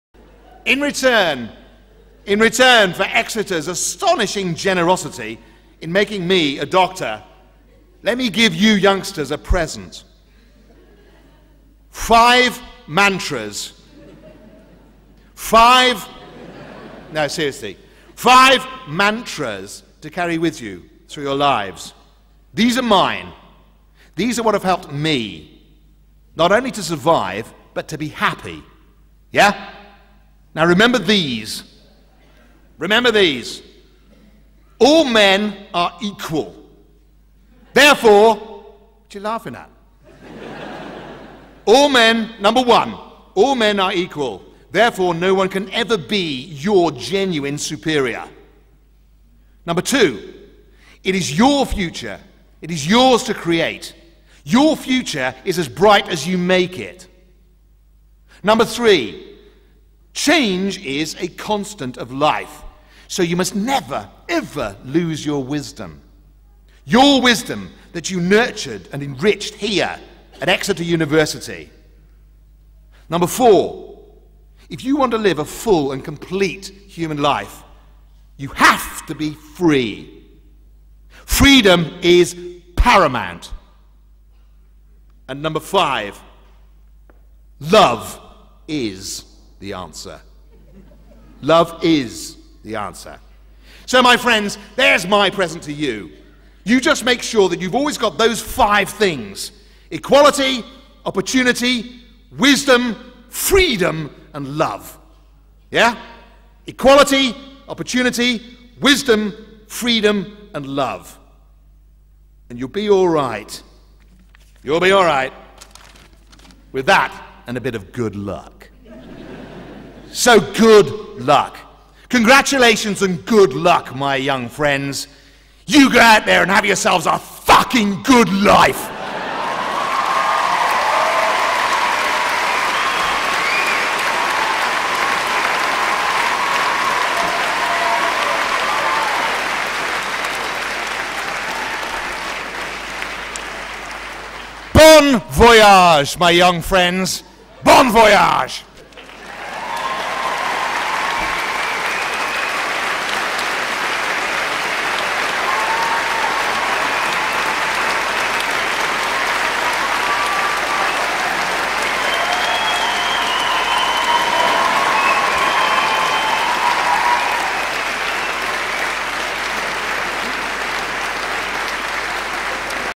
Rik Mayall - His 5 Mantra's For life
Speech when he got his doctorate, Wise words